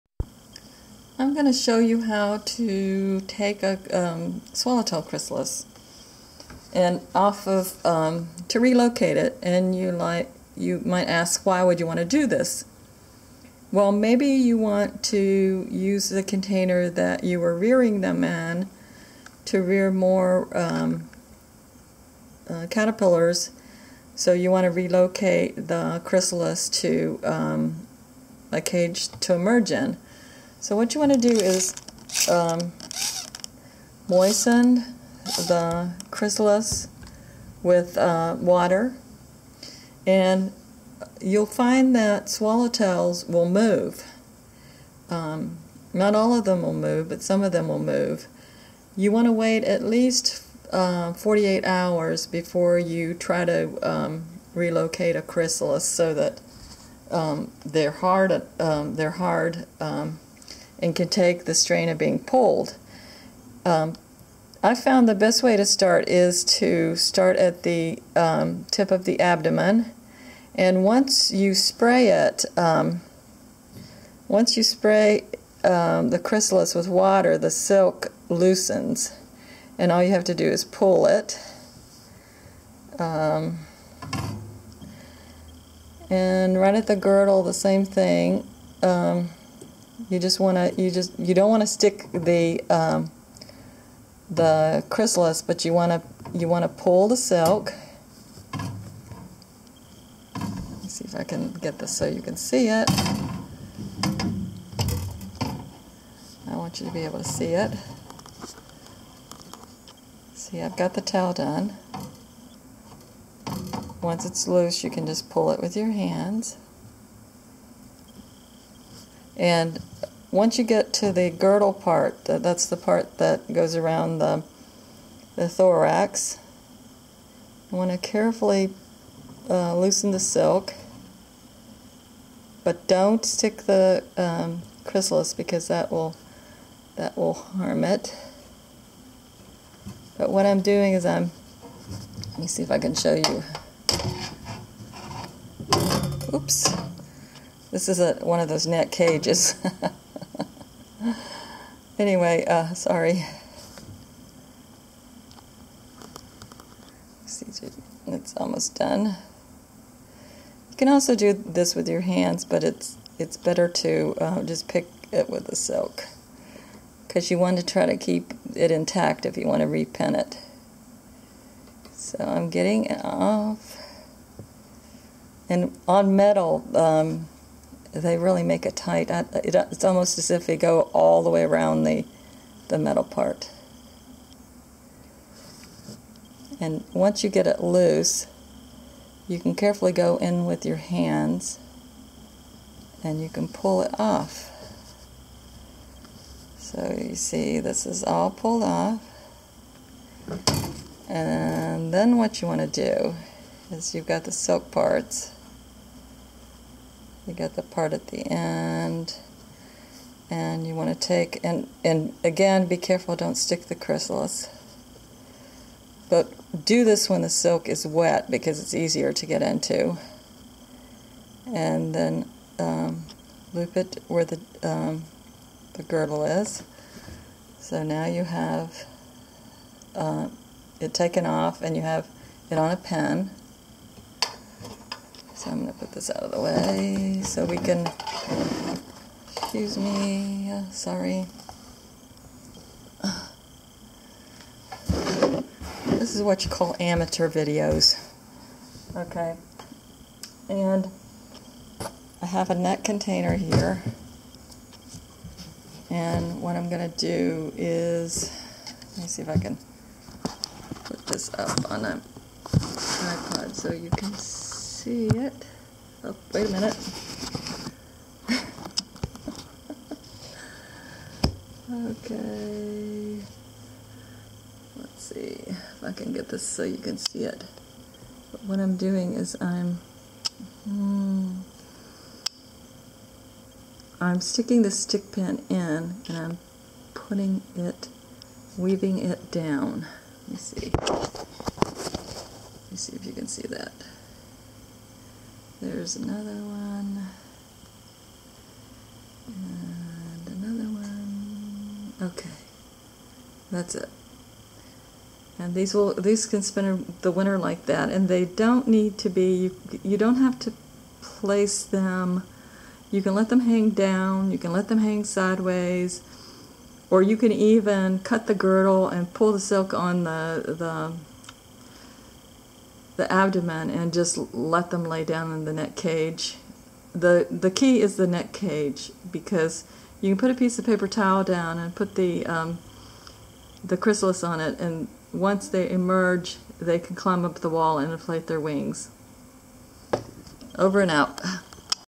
Removing and Pinning Swallowtail Chrysalis
Many species of butterfly chrysalises can be relocated by spraying the dried chrysalis (at least 48 hours).  The silk will loosen and can be lifted with a stick pin.  Once it is lifted and the pin is inserted into the silk, it can be pinned into a net cage.  Always make sure to leave the sharp end of the pin on the outside of the cage.  Pin from the inside of the cage, weaving down, in and out for stability.

Hobbyists are welcome to learn more about creating habitat and rearing butterflies and moths, please join our conservation group